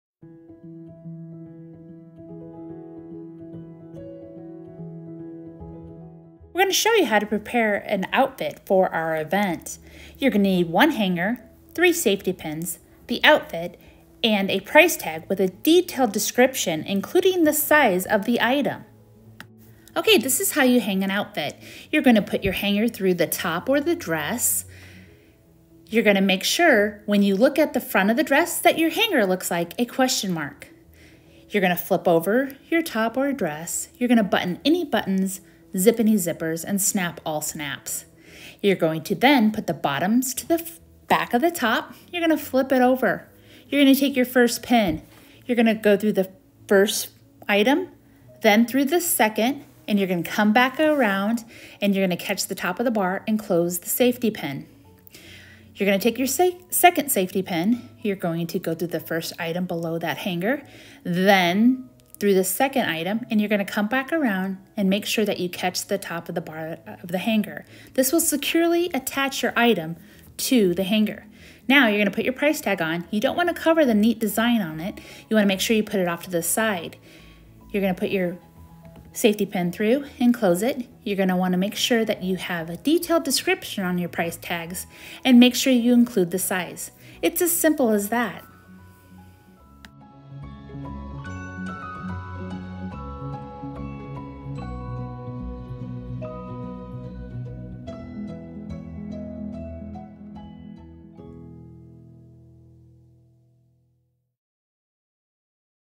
Hanging an Outfit
Here is a short video on how to prepare and hang an outfit for ReRUNS Kids Sale.